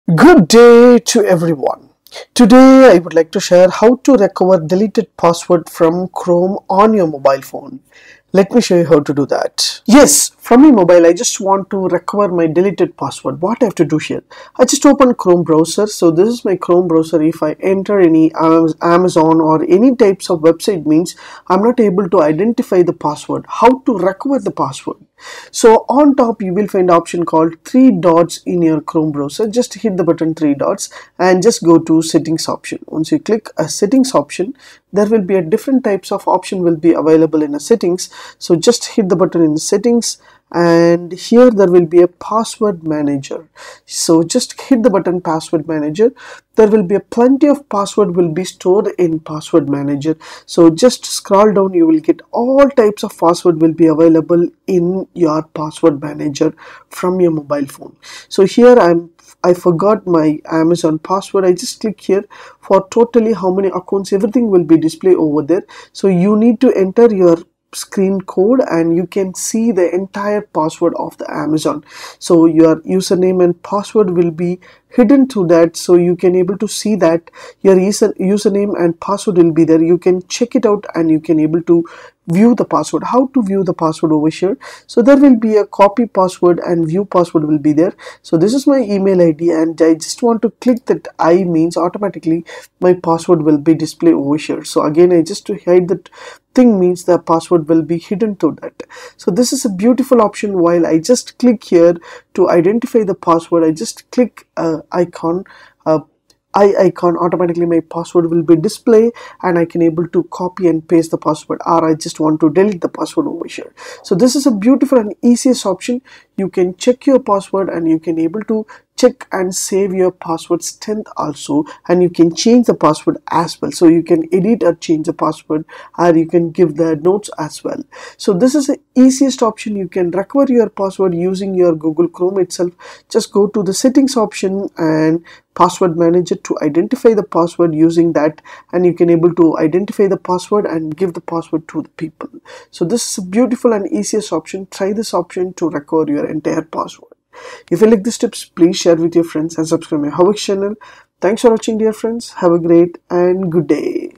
How to recover deleted passwords from chrome on mobile
This video tutorial will help you How to recover deleted passwords from chrome on mobile